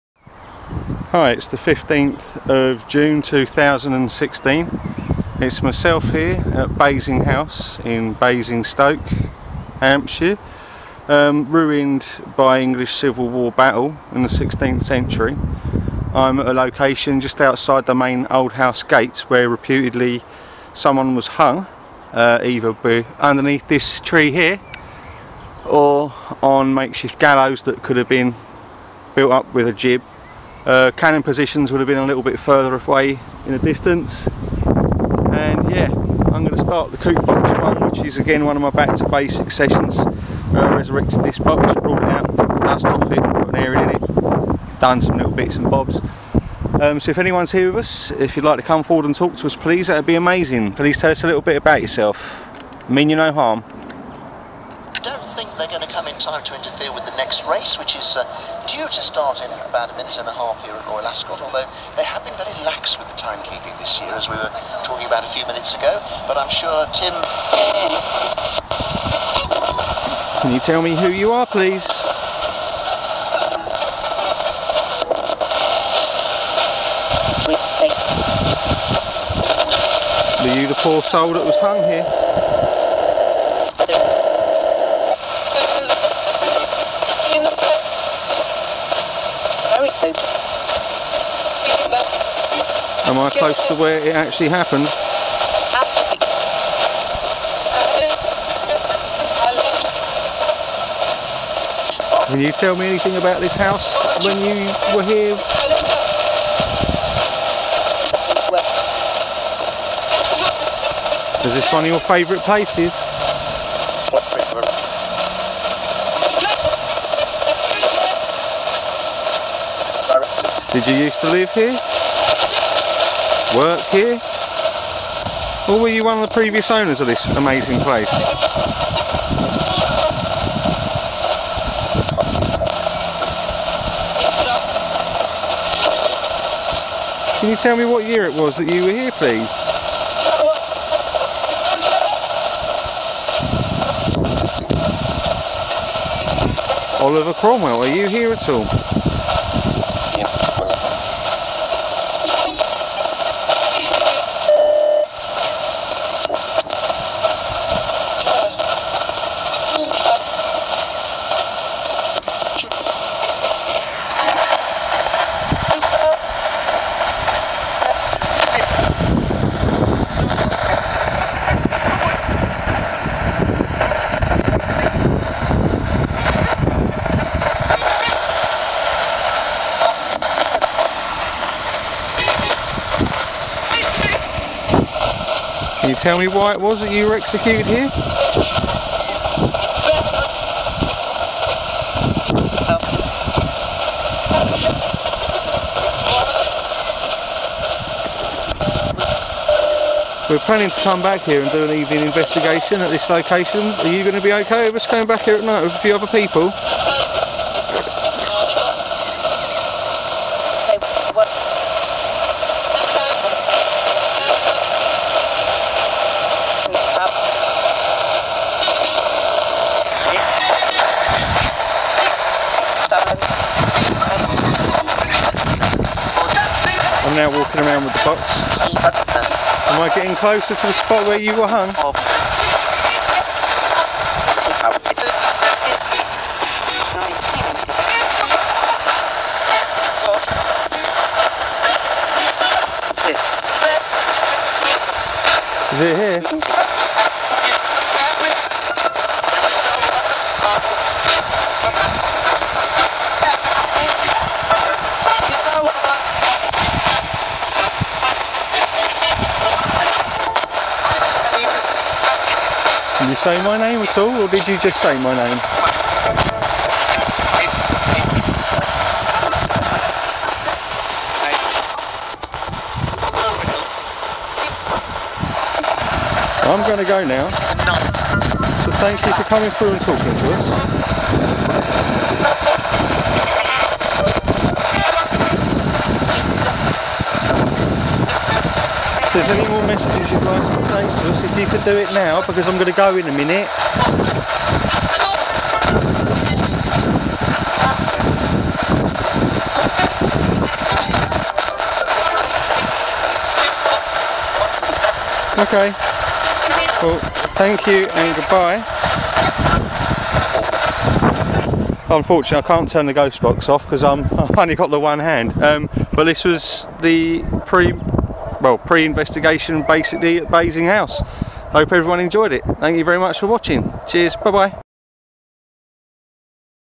15th June 2016 Basing House, CoopBox 1 session
Basing House, which was once a fortified Tudor house that was attacked and destroyed by Oliver Cromwell during the English Civil war.